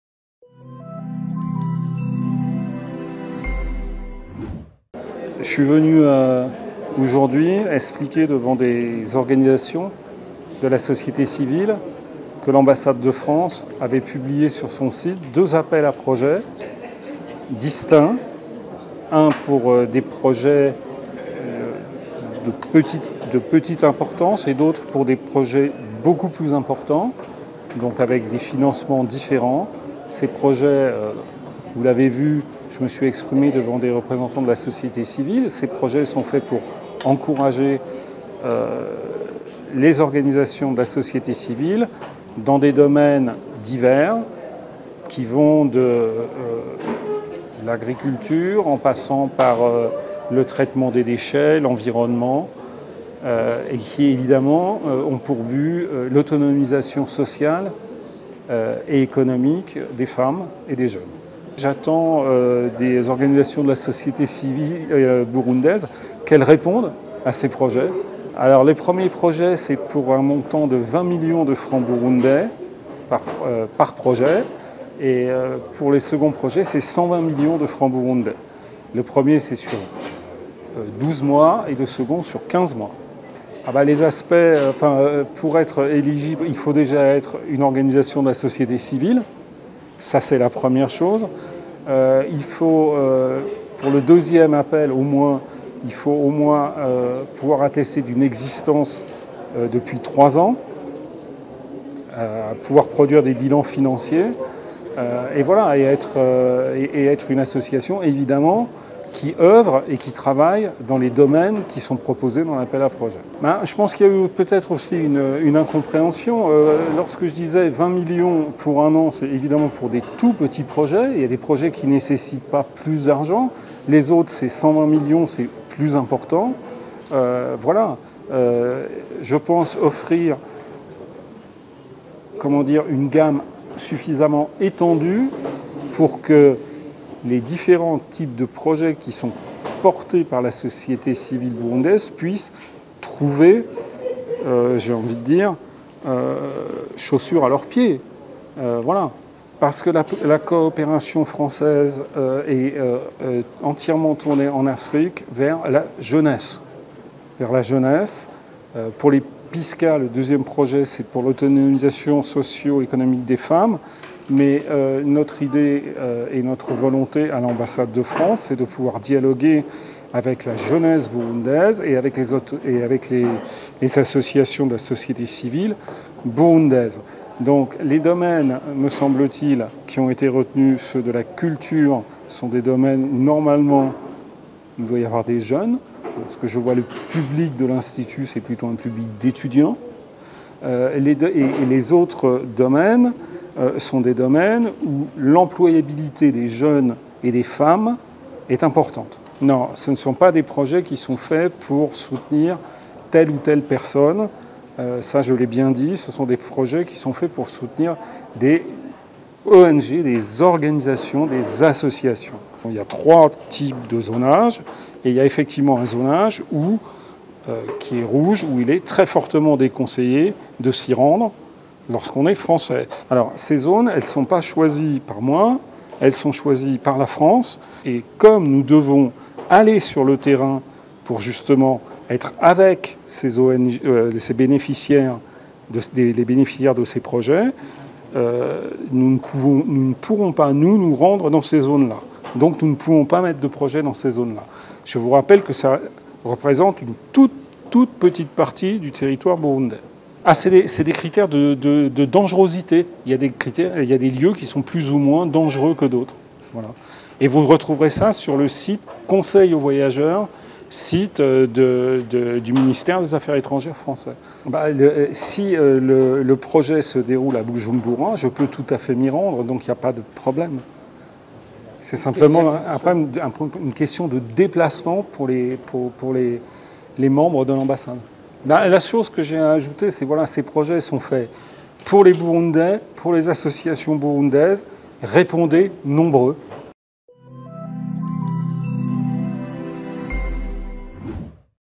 L’Ambassade de France au Burundi a procédé à l’ouverture d’un nouvel appel à projets
L’ambassade de France au Burundi a procédé ce Lundi 5 Juin 2023 à l’ouverture d’un nouvel appel à projets destiné aux porteurs de projets de la société civile burundaise. L’édition 2023 de l’appel à projets PISCCA permettra de subventionner une dizaine de projets, d’un montant maximum de 120 millions BIF chacun, s’inscrivant dans la « Promotion de l’autonomisation sociale et économique des femmes et des jeunes » (PASEF). La Date limite de dépôts de dossier est fixée au 26 juin à 12h

Suivez Gilles Ganivet, Conseiller de Coopération et d'action culturelle à l'Ambassade de France au